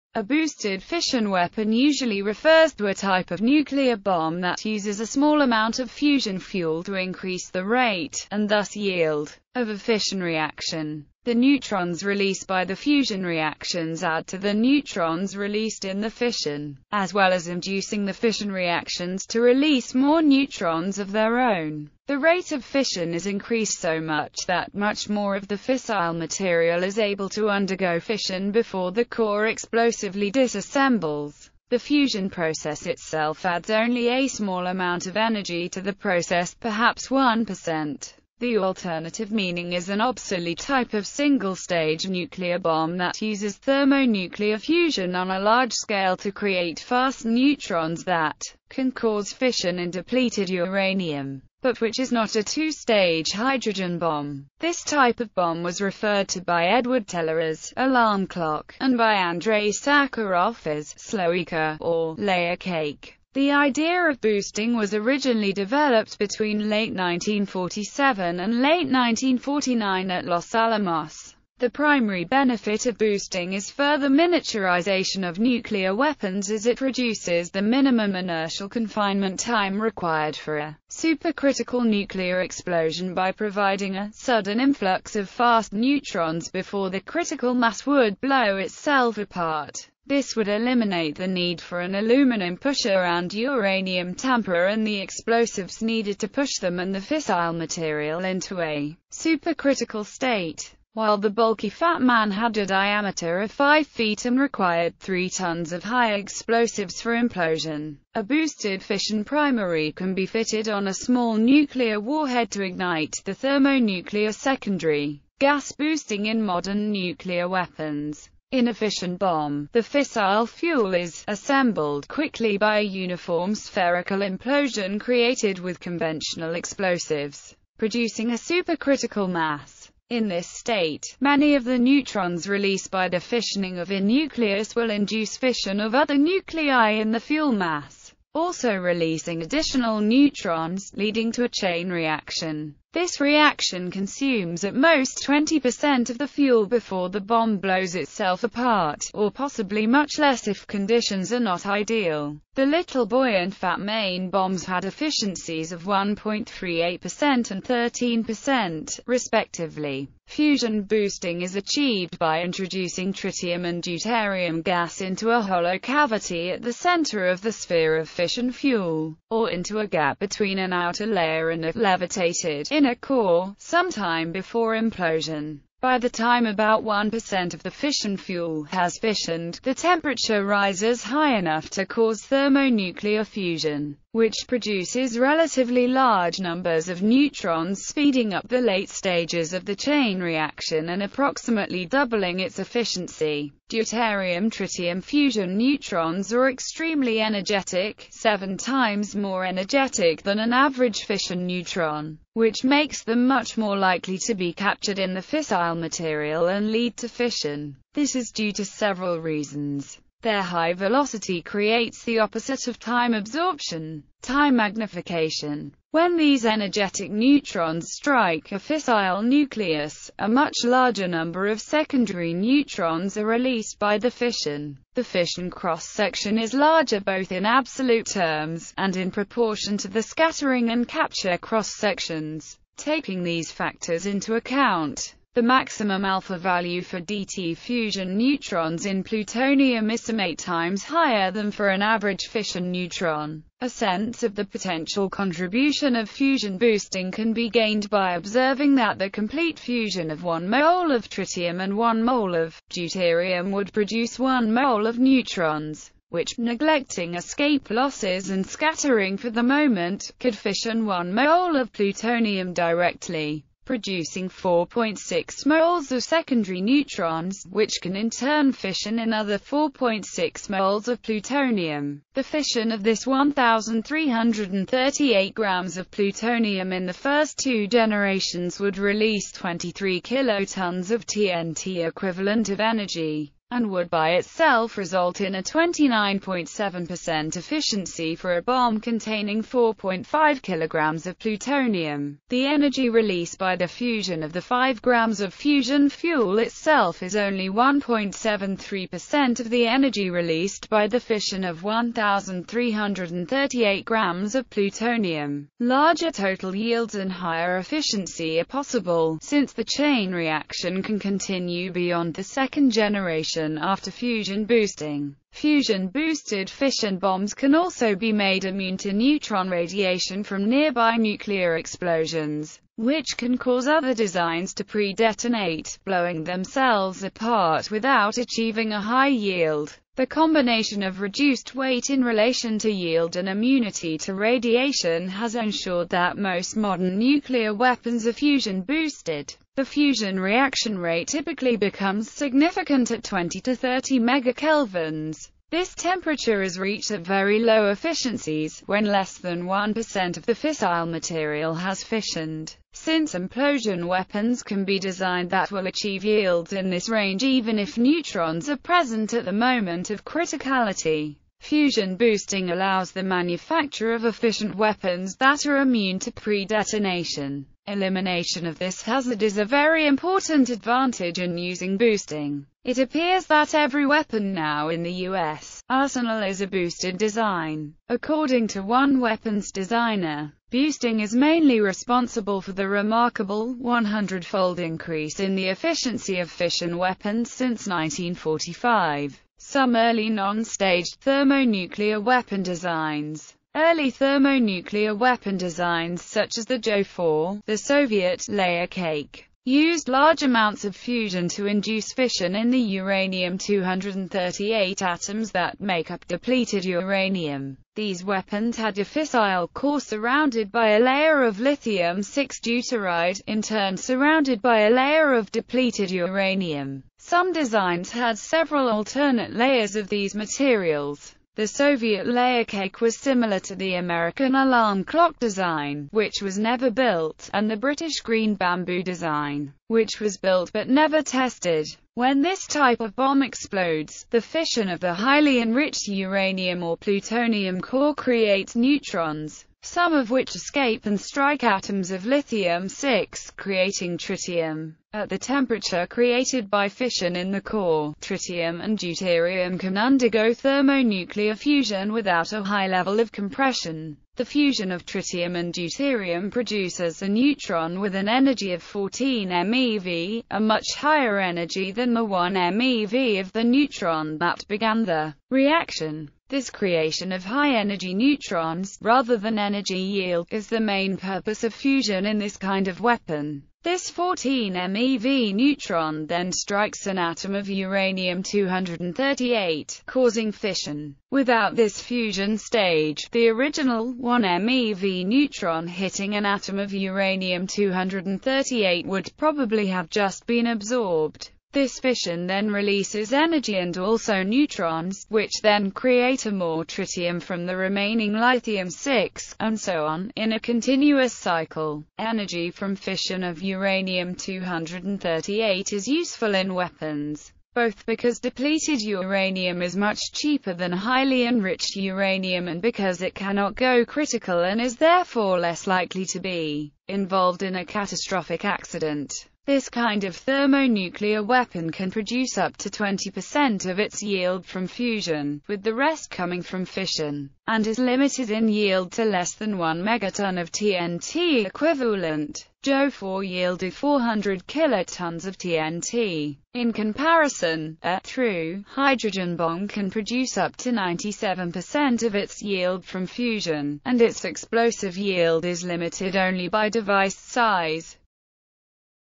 Boosted fission weapon
A boosted fission weapon usually refers to a type of nuclear bomb that uses a small amount of fusion fuel to increase the rate, and thus yield, of a fission reaction.The neutrons released by the fusion reactions add to the neutrons released in the fission, as well as inducing the fission reactions to release more neutrons of their own.